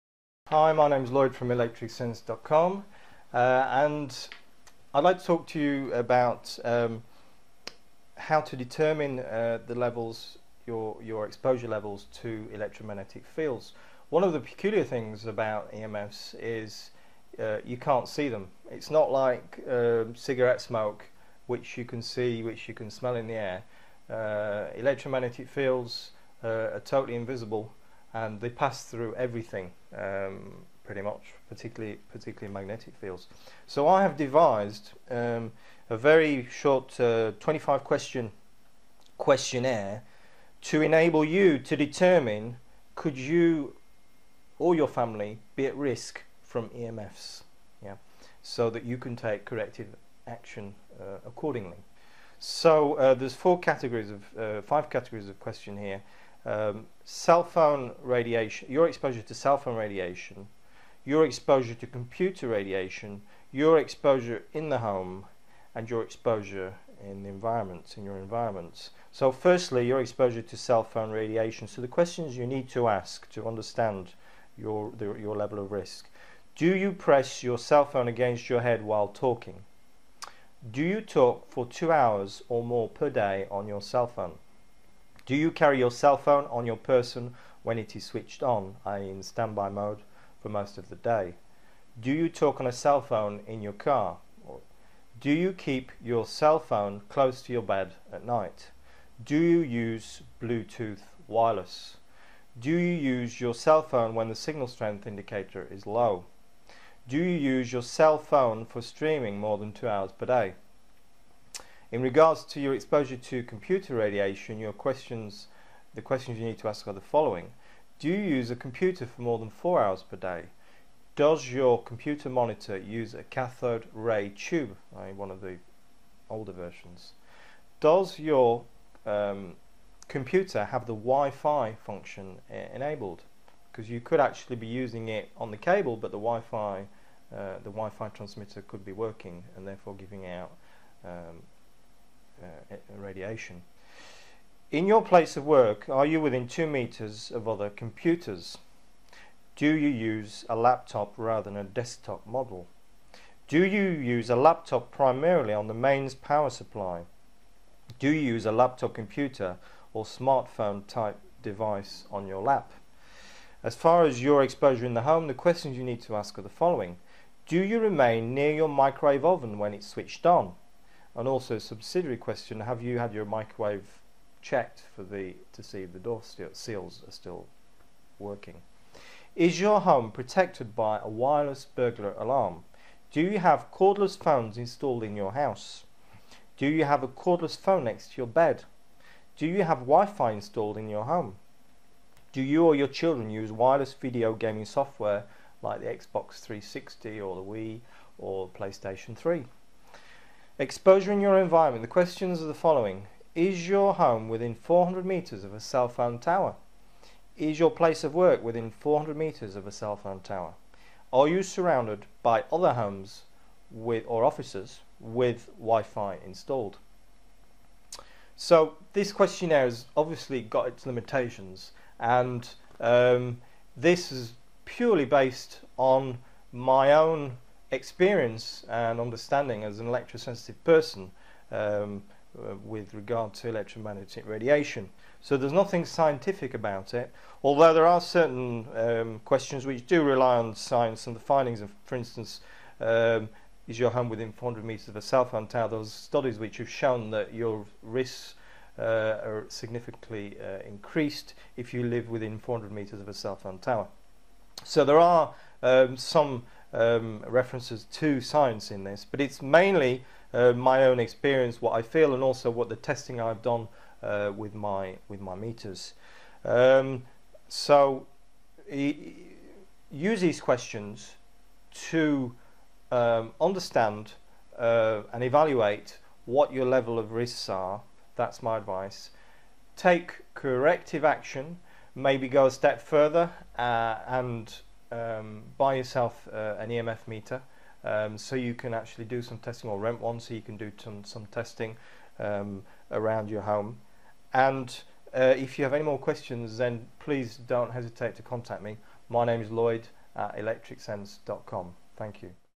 How To Determine Your Exposure To Electromagnetic Fields
EMFs or electromagnetic fields or electromagnetic radiation is all around us. As we become more and more dependent on wireless communication as used by portable electronic devices such as smart phones and laptop computers,  the wireless infrastructure which is needed to facilitate the operation of these devices becomes more and more developed.

In our homes more and more devices use electromagnetic energy to function, microwave ovens, electric blankets, video game consoles (Playstation and the like) and also just outside our homes smart meters are being introduced, cell phone towers are ever present.

This growth of electrosmog is invisible.

I have developed a short questionnaire to enable you to evaluate your exposure to these electromagnetic fields. in all there are 25 questions split into the following four categories:

exposure to cellphone radiation

exposure to computer radiation

exposure in your home

exposure in your environment

this questionnaire is a good starting point to gain some understanding as to the electromagnetic fields that are present in your daily life. If you would like to take this a step further than you can very easily measure these levels of electromagnetic radiation by using an EMF gauss meter.

More than 2000 studies now document the adverse health effects of electromagnetic fields.

You really need to take action today to protect you and your family.